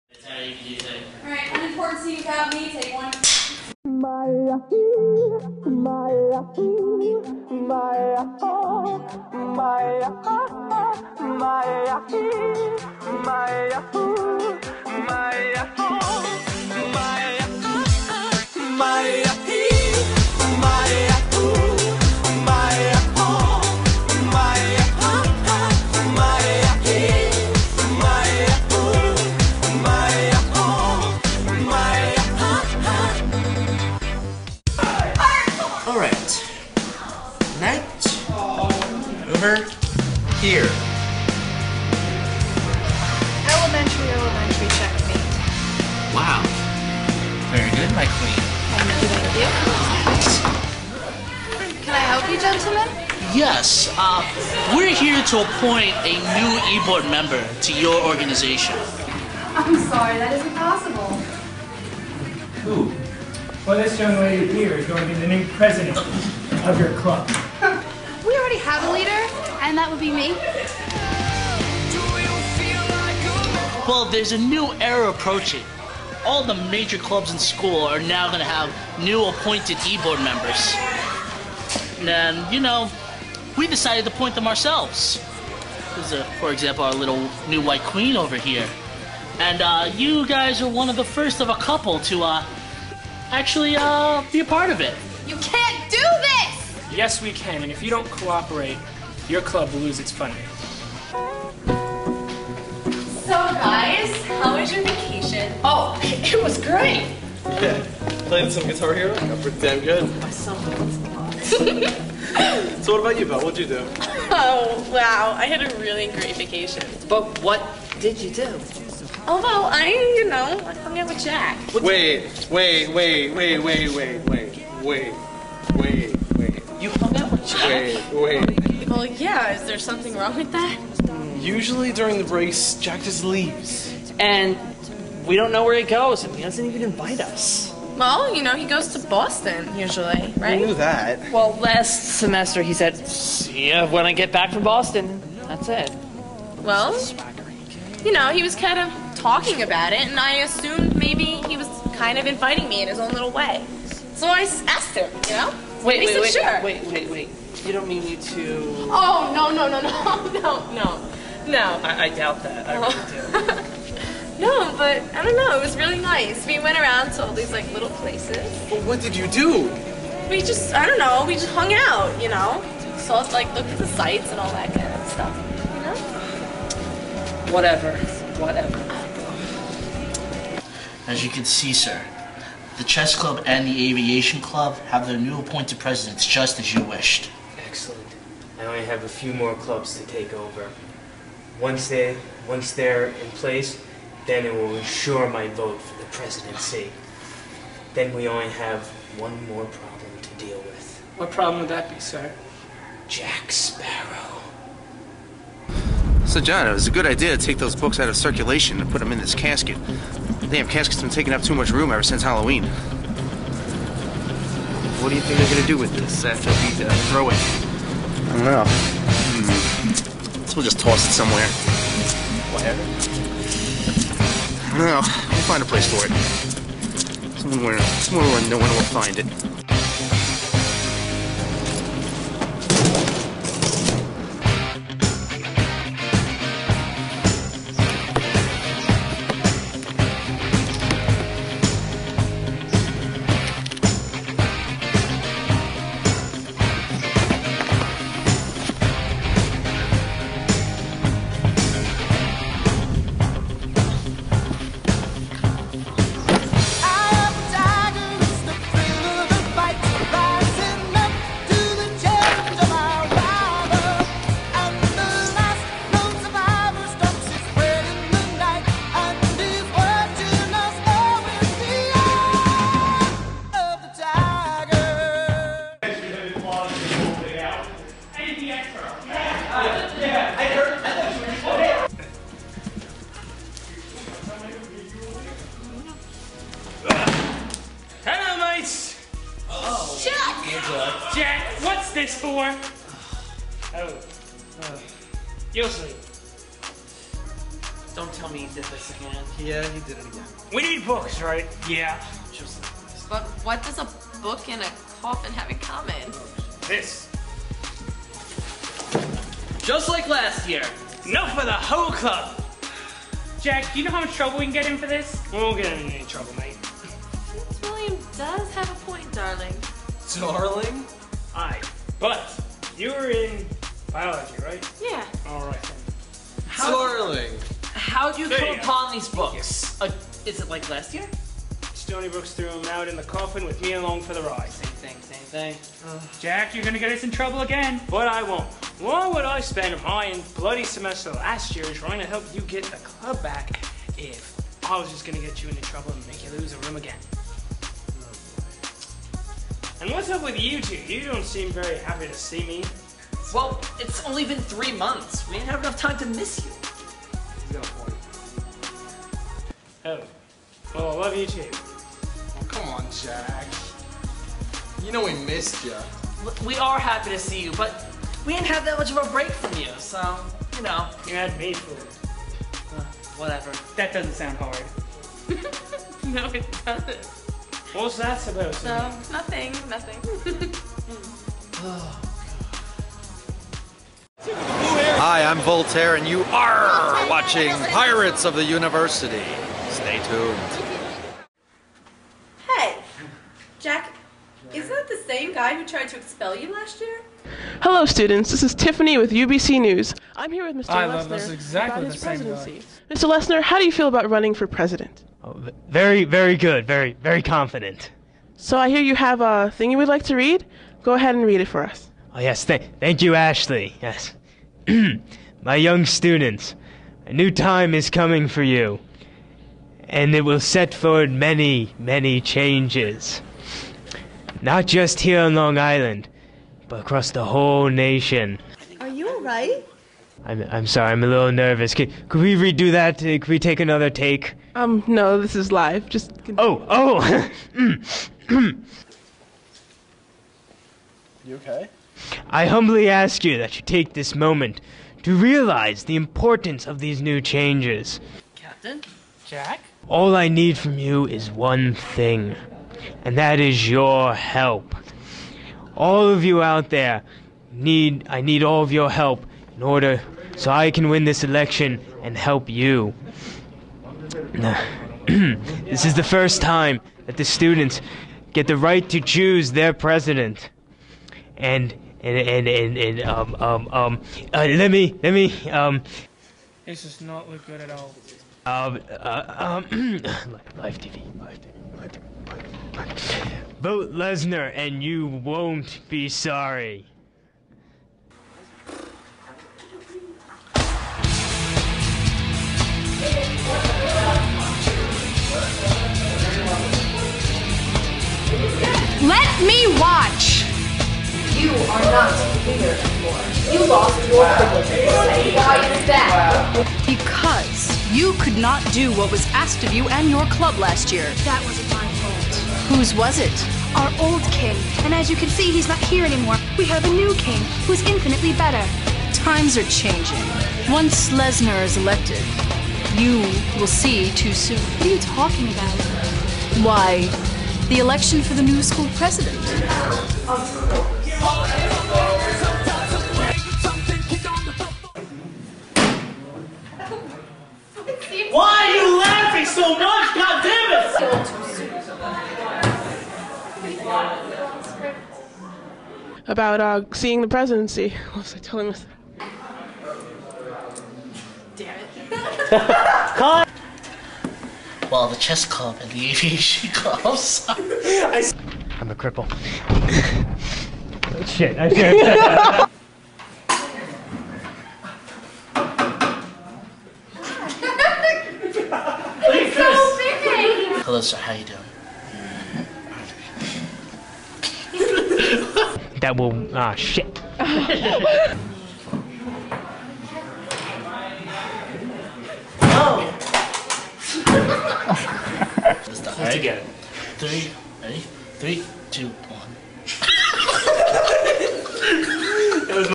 SEASON PREMIERE!!!! of Yacht Man's Chest
Ep01 - The New Queen

The spring semester has finally arrived, as changes are apparent for the Stony Brook campus.